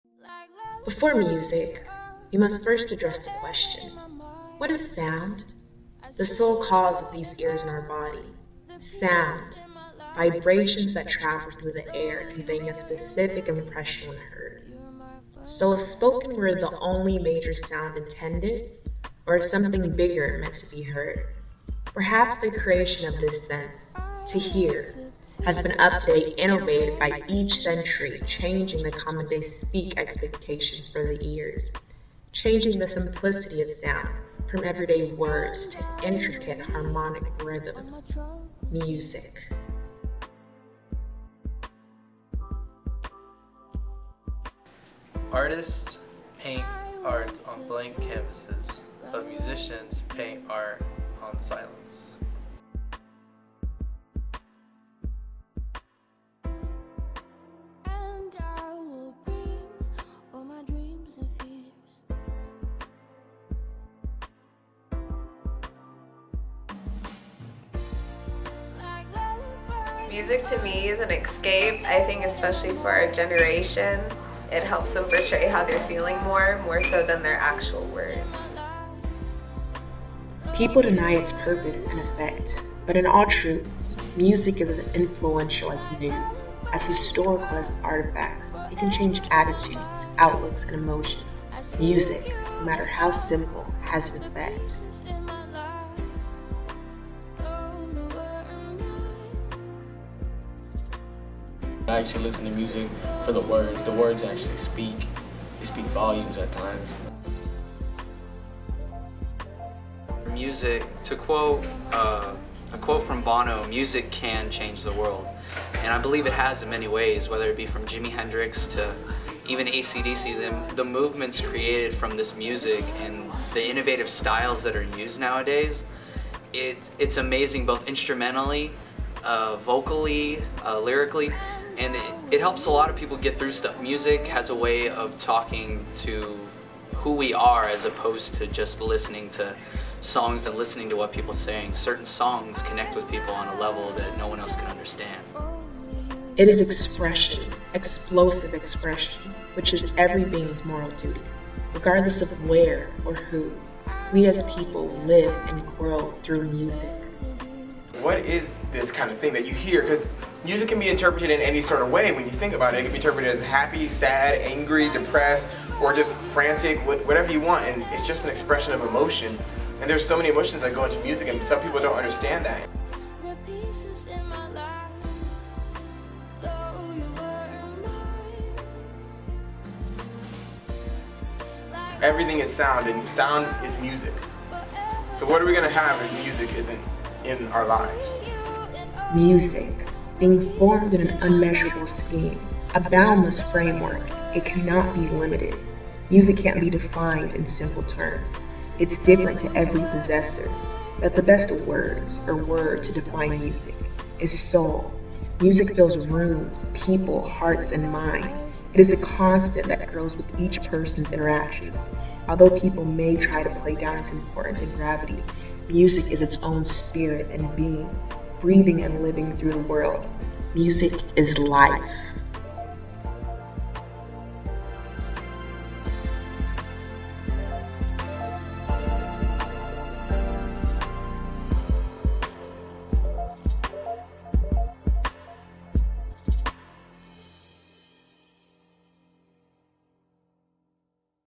Golden Panorama of Music
Student Petra Ihezie created a video displaying her interpretation of music.